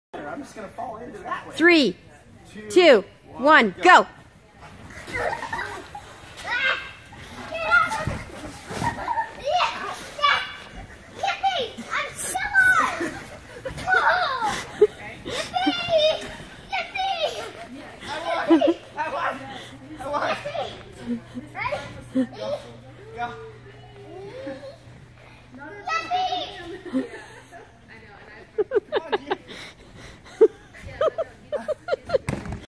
Bouncy Donkey Race
Hopping bouncing donkey race down the trampoline.